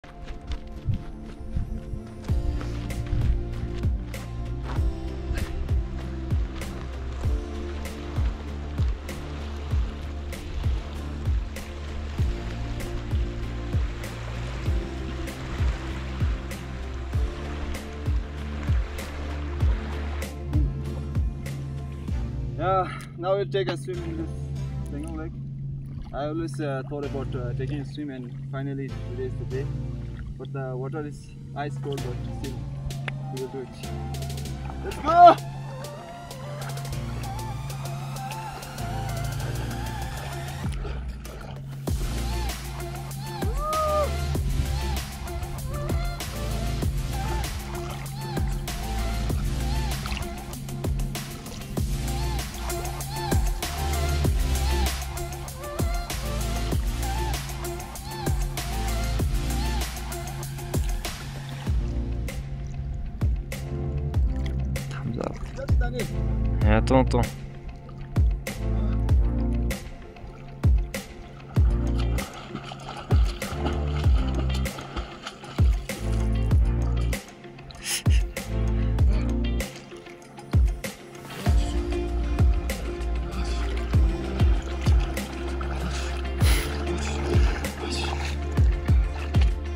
Ice bath at Pangong lake | Ladakh
After many years finally I got a chance  to take an ice bath at Pangong lake. It was during summer time July but the water was still ice cold.